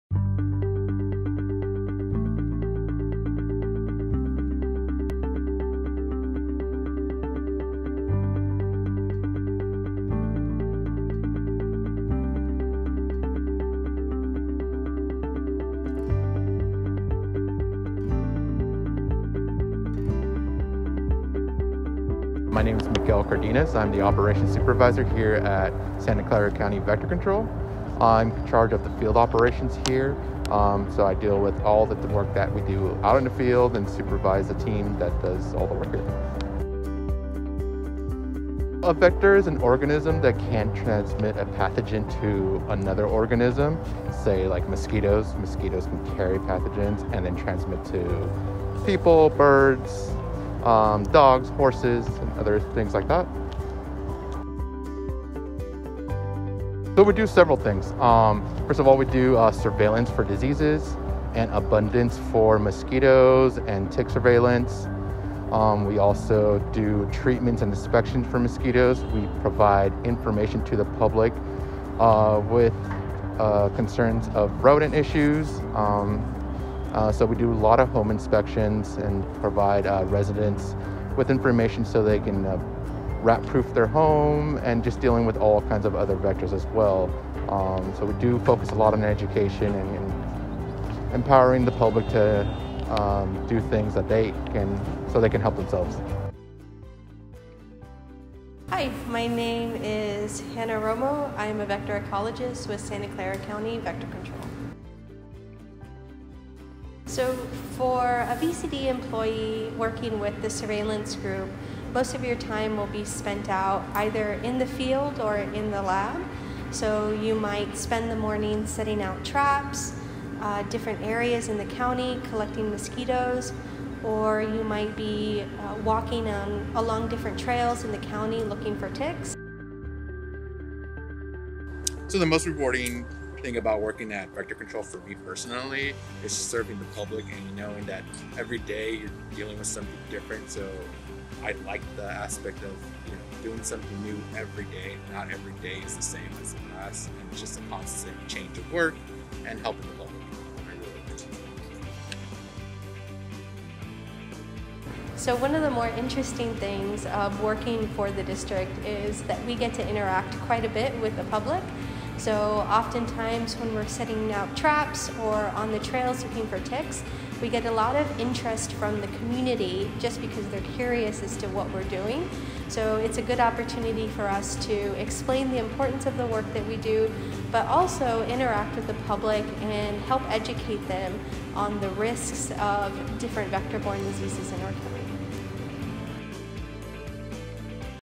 Get to know us!
Meet part of our team and learn what the County of Santa Clara Vector Control District is all about.